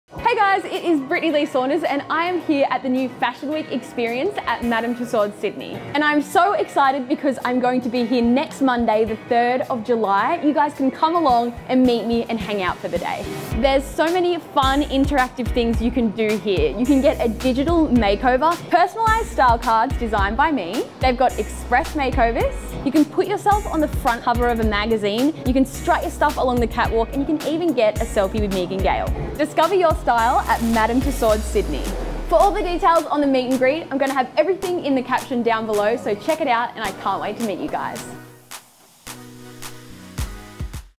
DISCOVER YOUR STYLE at Madame Tussauds Sydney this school holiday
DISCOVER YOUR STYLE this school holidays at Madame Tussauds Sydney’s pop-up fashion and beauty event in our all-new Fashion Week experience plus meet beauty blogger Brittney Lee Saunders.